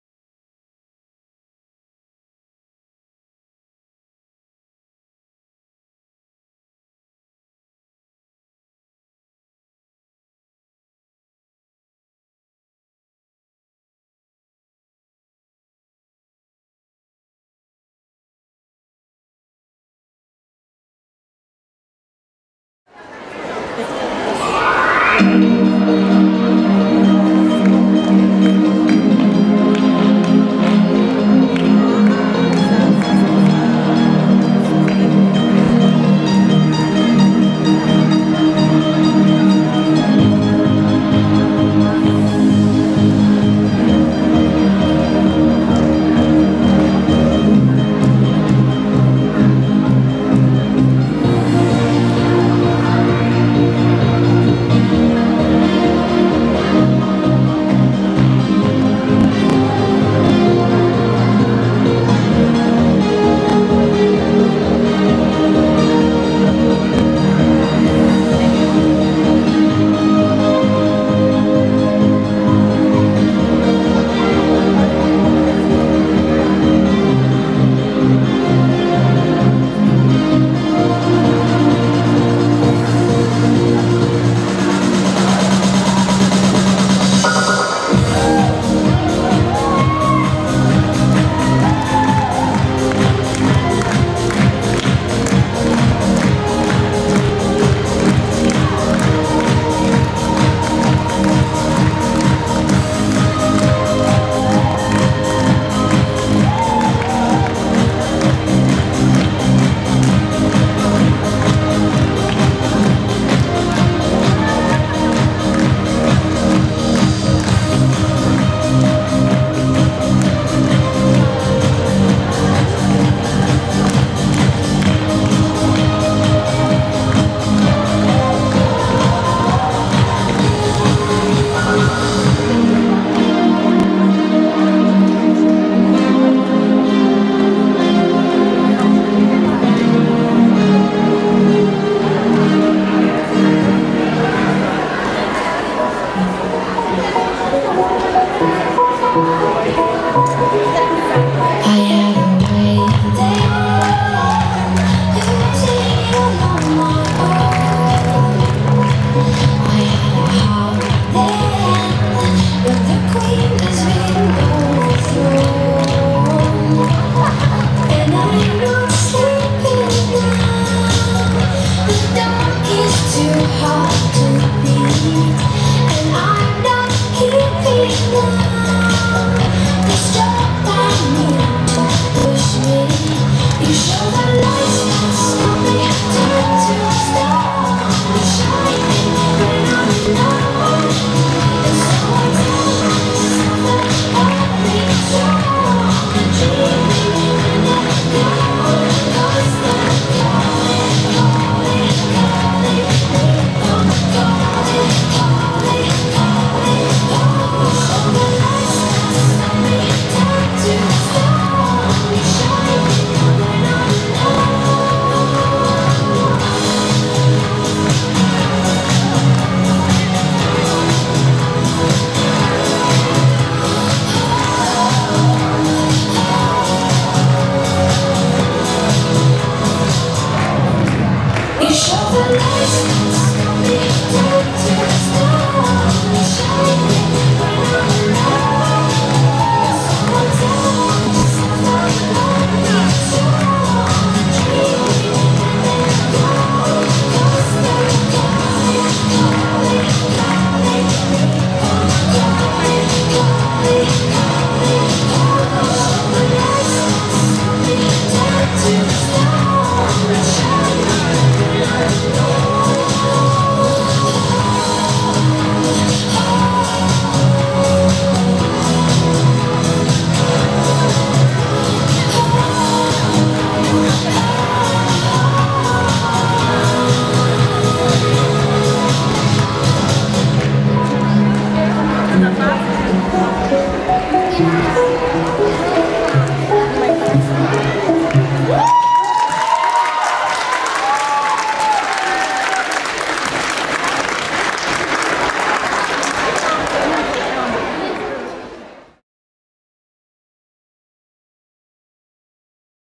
7. 4. 2013 - Děti v akci / 21 / Wild Cats Benešov / Světlo života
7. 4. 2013
DĚTI V AKCI
Přehlídka tanečních a sportovních týmů.

Organizátor:
Dům dětí a mládeže Jižní Město

Finanční podpora:
Hlavní město Praha